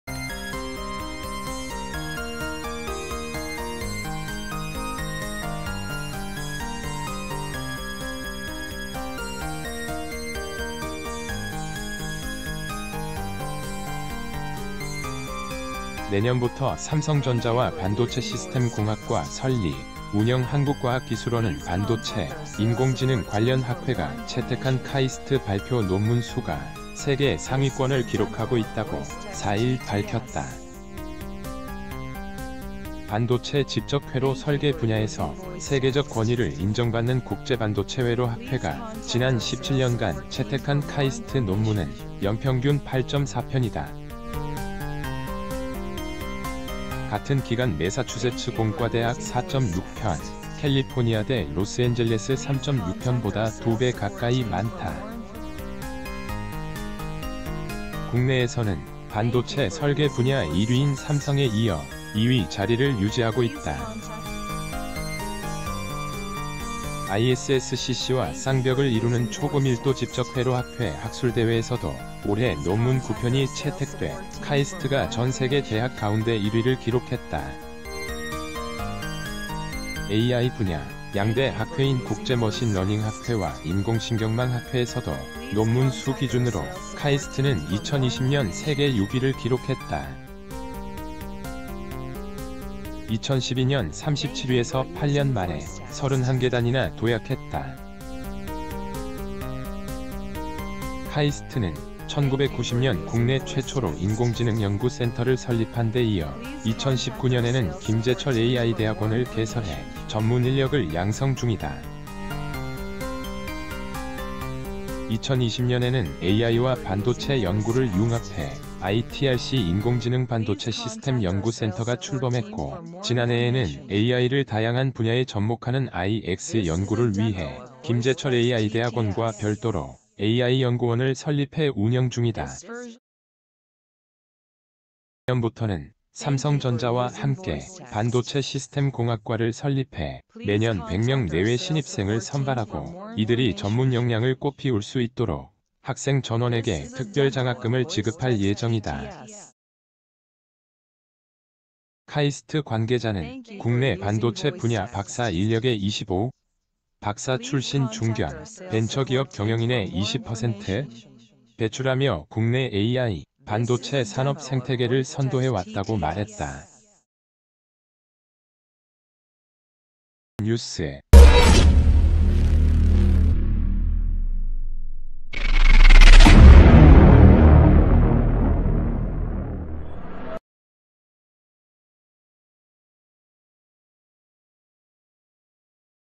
KAIST "반도체·AI 생태계 선도…논문 수 세계 상위권"
,han woong yeom,conference,lecture,taekjip ha,hongkun park

KAIST "반도체·AI 생태계 선도…논문 수 세계 상위권" 내년부터 삼성전자와 반도체시스템공학과 설립·운영